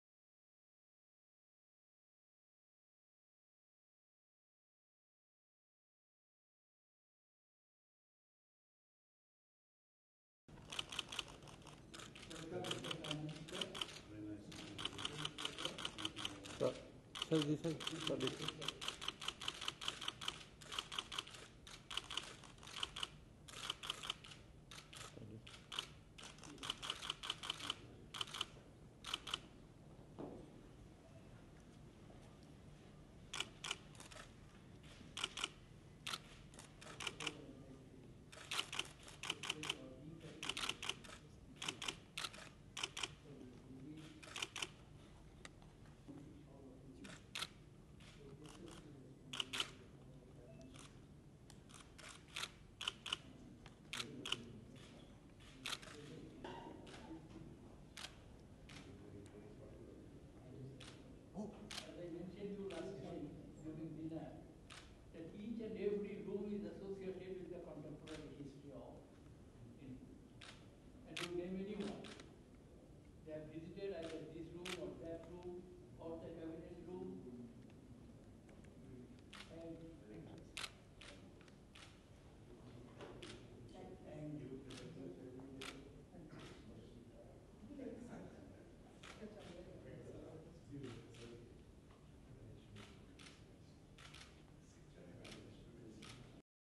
Mr. Muhammad Nawaz Sharif, Prime Minister of Pakistan called on President Mukherjee
Mr. Muhammad Nawaz Sharif, Prime Minister of the Islamic Republic of Pakistan called on President Mukherjee at Rashtrapati Bhavan today on May 27, 2014.

Welcoming Mr. Muhammad Nawaz Sharif, the Prime Minister of Pakistan, the President expressed appreciation for the gracious response of the Pakistan Prime Minister to the invite from our new Prime Minister, despite the short notice. He reiterated that it has always been India’s desire to build peaceful, friendly and cooperative ties and this will continue to be the policy of the new Government. Prime Minister Nawaz Sharif said he considers it a personal privilege to be in India. The Government in both the countries have a strong mandate and that this is a historic moment for both countries to move forward in the relationship and remove the mistrust. President Mukherjee said efforts should be made to open up trade and economic co-operation. This is an opportunity and a historic moment which should not be missed.